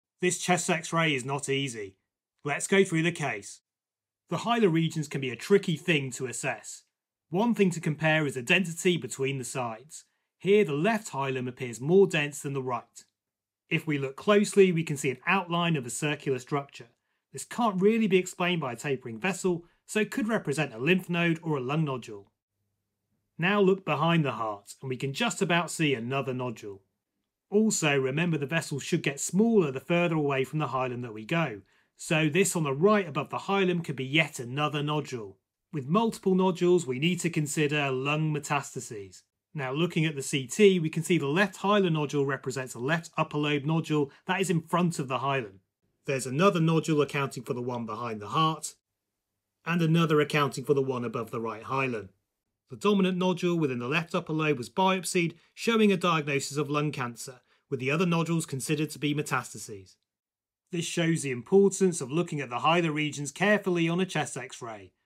Chest X-Ray breakdown: assessing the hilar regions
A female smoker in her 50s presents with a few week history of cough. What does the Chest X-Ray show?

LUNG CANCER AND METASTASES

👨🏽‍💻This is a tricky X-Ray with some subtle findings:

1️⃣Left hilum: compare the two hilar regions - the density of the left is increased compared with the right. Looking closely you can just make out a circular structure on the left - small circular densities are ok and can represent end on vessels but larger ‘lumps’ like this can represent lymph nodes or tumour.

Remember a frontal X-Ray is a 2D representation of a 3D structure so an abnormality at the hilum may actually be in front of the hilum as it is here - the abnormal hilum actually relates to a left upper lobe nodule

2️⃣ Don’t forget to look behind the heart and check left with right - here there is a nodule on the left confirmed with CT

3️⃣ The regions just above and below the hilum are common places for missed lesions - here there is a subtle nodule above the right hilum. This would have been a tough ask if this was the only abnormality on the film but with the other abnormalities this has to be considered suspicious

Putting everything together here the differential includes
1️⃣Lung cancer and associated tumour nodules
2️⃣Metastases from a separate primary

A CT guided biopsy here confirmed number 1️⃣

The lesson here is to really piece apart the hilar regions on every film you look at

✅Patient consent obtained